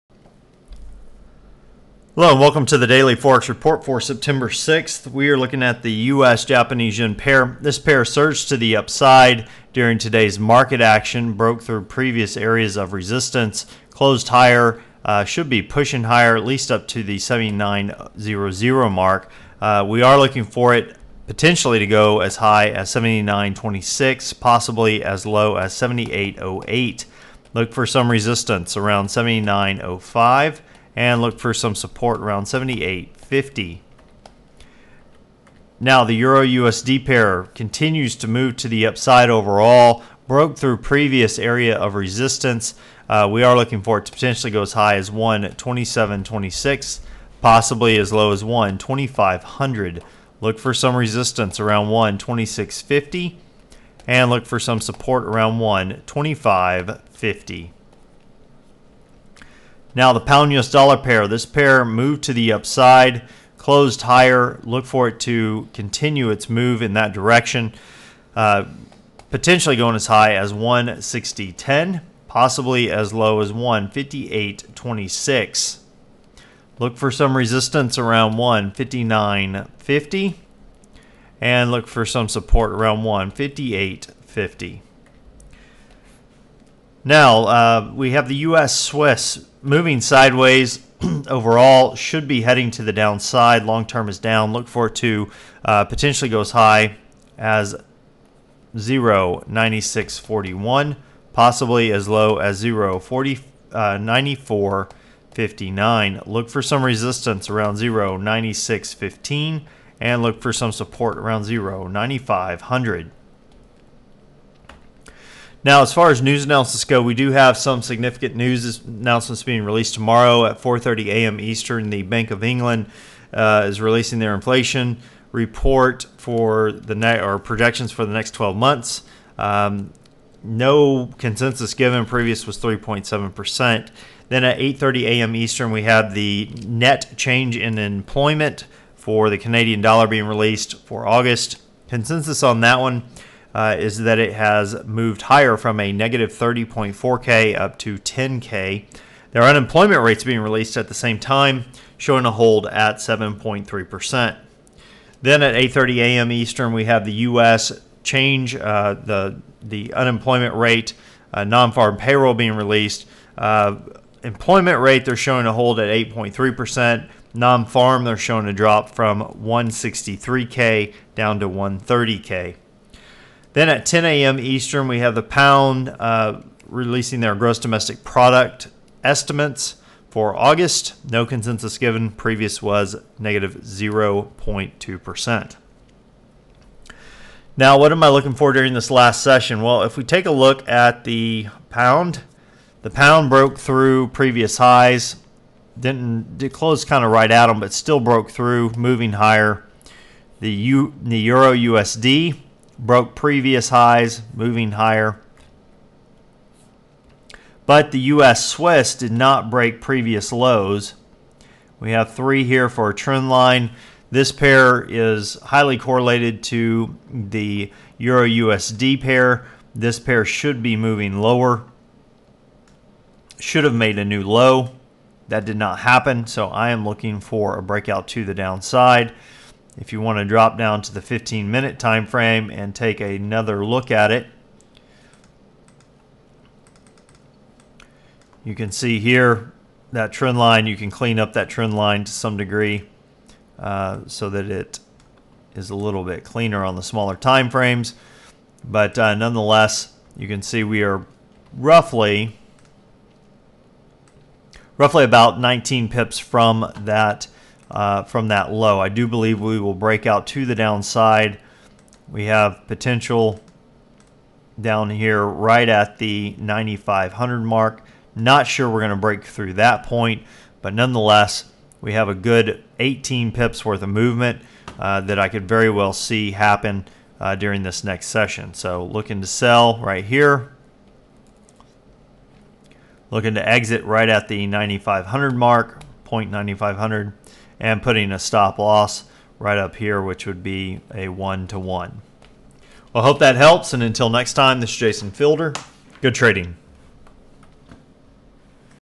USD/CHF Correlation Trade...
The USD/CHF did not move as much as it should have with today market action. So, we are looking to profit from this, to find out how watch the video...